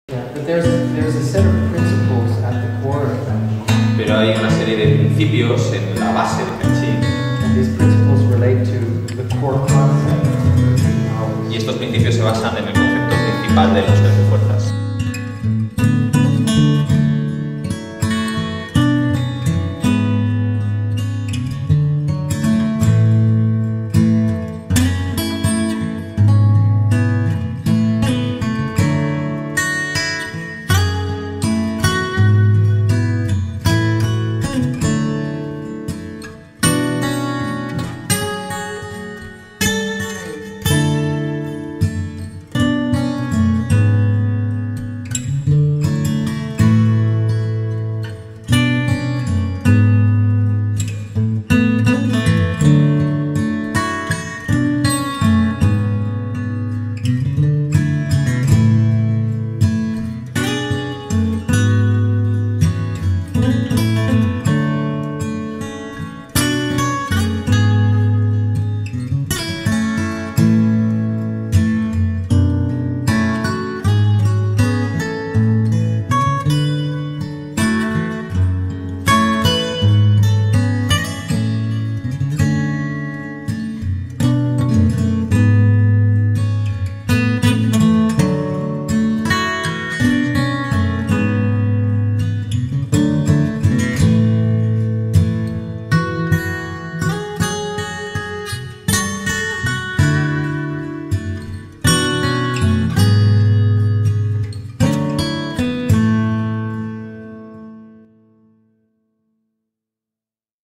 Sam Masich en Salamanca, Los 13 Poderes del Tai Chi Chuan
Primer taller del Maestro  Sam Masich en Salamanca (marzo de 2016), organizado por el Club Deportivo Guan Yu Salamanca y el Instituto Movimiento y Salud, con la inestimable colaboración de la Asociación Bagua de Cabrerizos.
Mi más sincero agradecimiento al Maestro Sam Masich, a Yanira Masich, a Javier Arnanz y a todos los asistentes por haberlo hecho posible.

Foografia y video originales: Marina Lorenzo.
Montaje para YouTube: David Gómez.